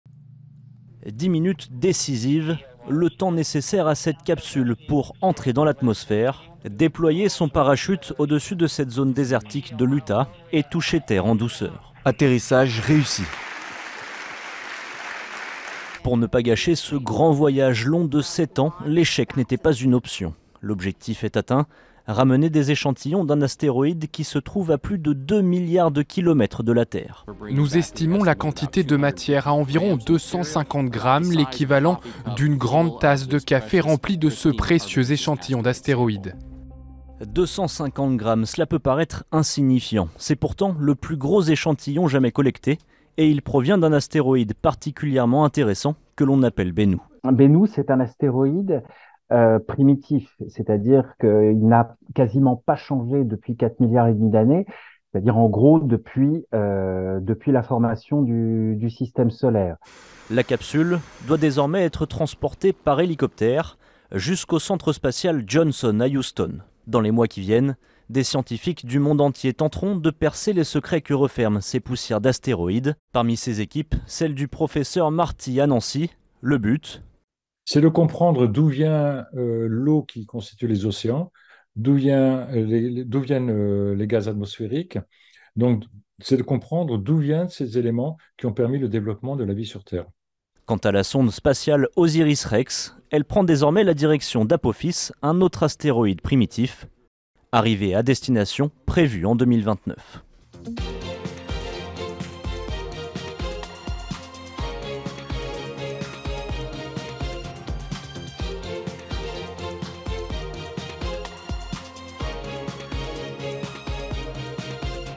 Des échantillons d'astéroïde pour comprendre l'origine de la vie sur Terre
Après 7 ans de voyage à travers l'espace, la sonde OsirisRex lancée par la NASA chargée de prélever des échantillons d'astéroïdes vient d'atterrir dans le désert de l'Utah (États-Unis). Les scientifiques espèrent en apprendre davantage sur la formation de la Terre…